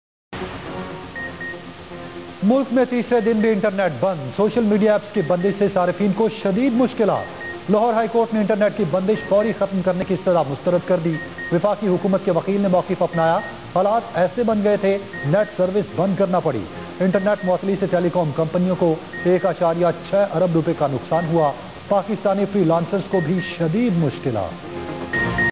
Mobile internet service suspended in different cities of the country, loss of 1.6 Bbilion to telecom
Mobile internet service suspended in different cities of the country for the third day, loss of 1.6 billion to telecom companies